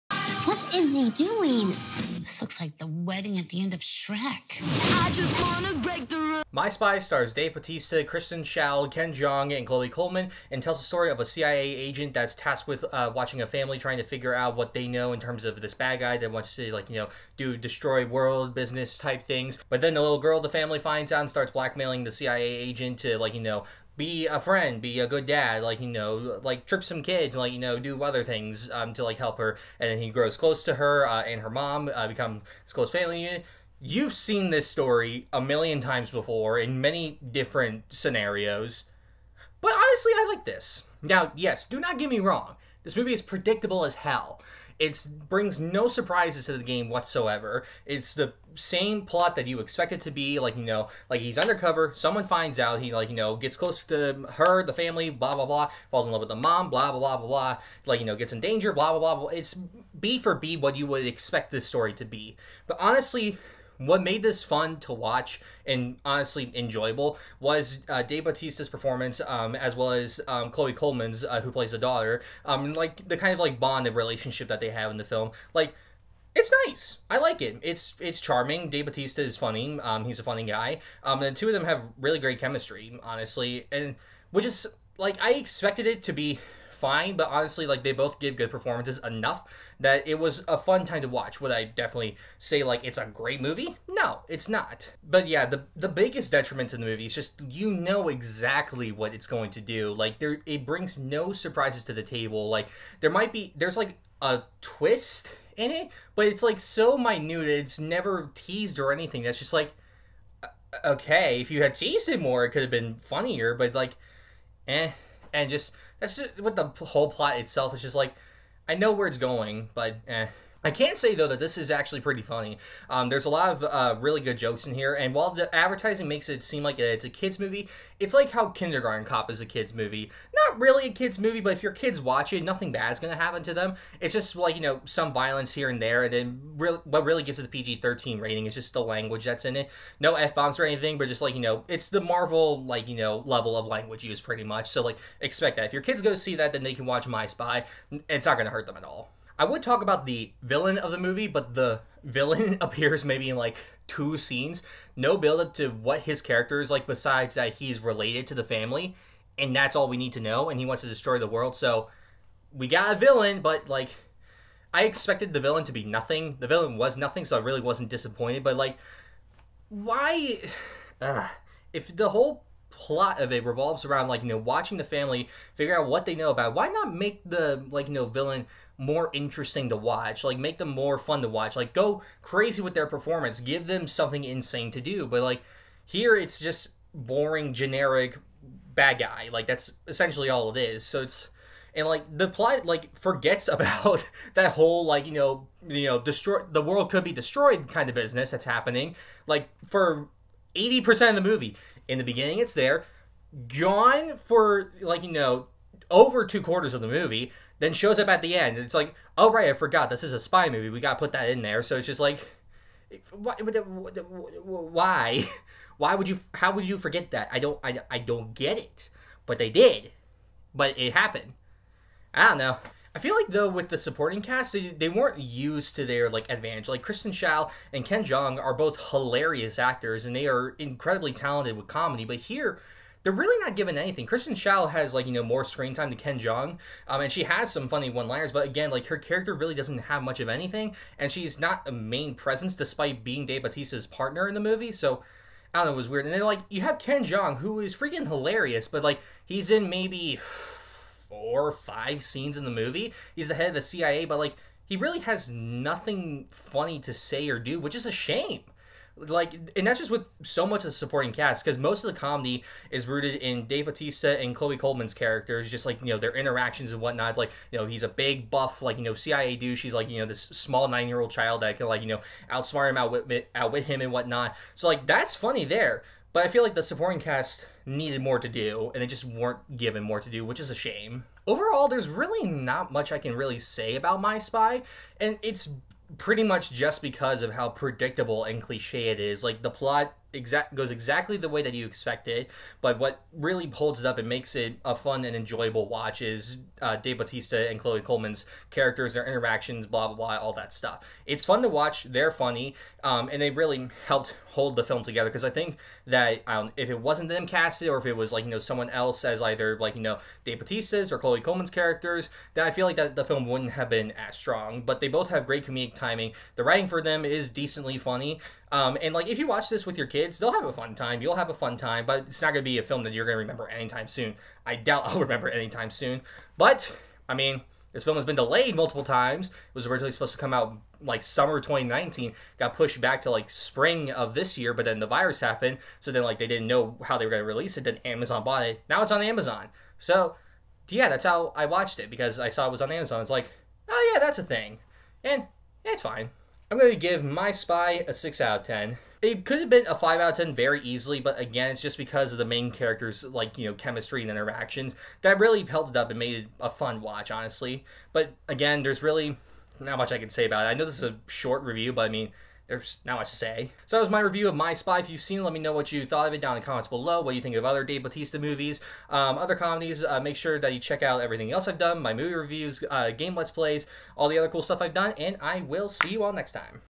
My Spy | movie review
My Spy, the constantly delayed Dave Bautista family/action comedy, is finally here thanks to Amazon!! Is it worth the wait? Find out!!

Check me out on Instagram!

Check me out on Twitter for the occasional tweet!